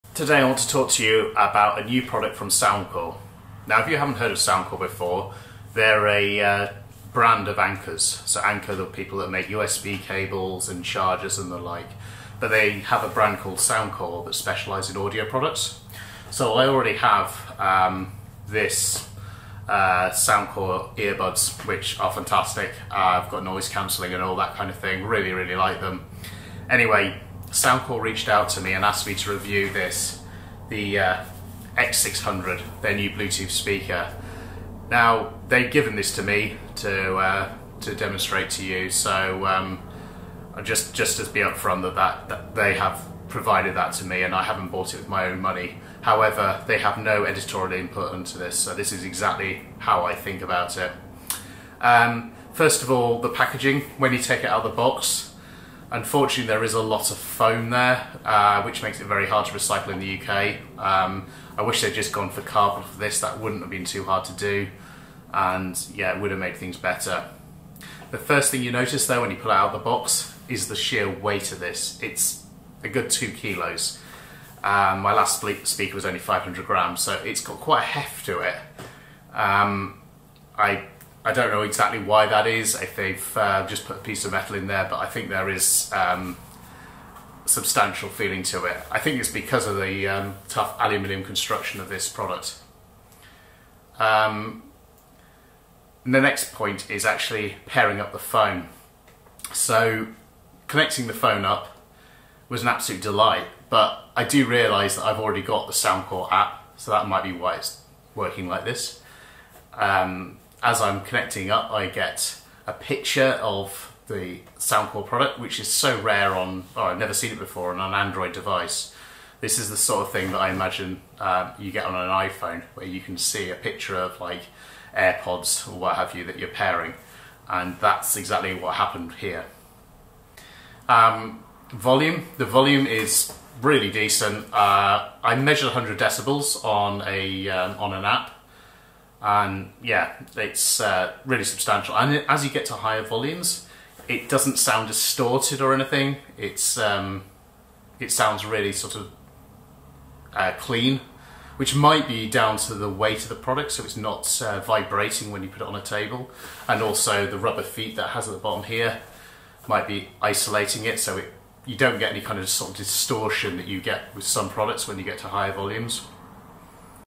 Soundcore X600 (Bluetooth Speaker) Review
This is the new Bluetooth Speaker from Soundcore.  In this video I try it out and give my views on it.